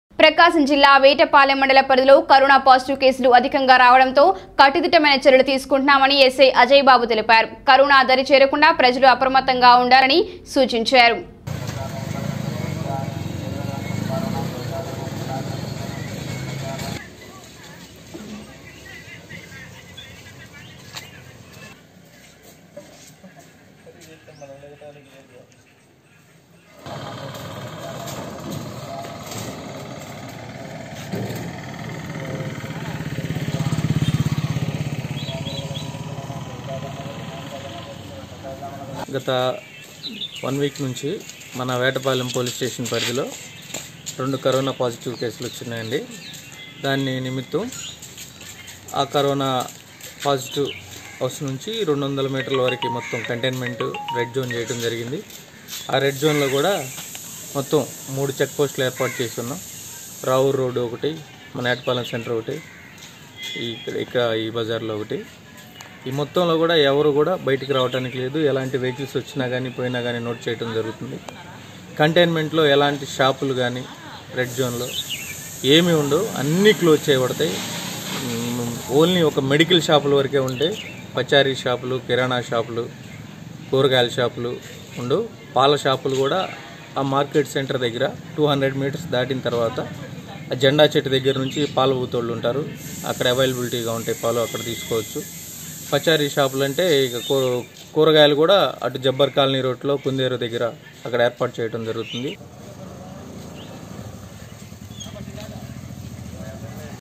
ప్రకాశం జిల్లాలో రోనా పాజిటీవ్ అధికం ప్రజలు అప్రమత్తంగా ఉండాలి: ఎస్ఐ ||VS9NEWS
Watch:
వేటపాలెంలో
ప్రజలు అప్రమత్తంగా ఉండాలి: ఎస్ఐ

ప్రకాశం జిల్లా వేటపాలెం మండల పరిధిలో కరోనా పాజిటీవ్ కేసులు అధికంగా
రావడంతో కట్టుదిట్టమైన చర్యలు తీసుకుంటున్నామని ఎస్ఐ అజయ బాబు తెలిపారు. కరోన దరి చేరకుండా ప్రజలు అప్రమత్తంగా ఉండాలని సూచించారు.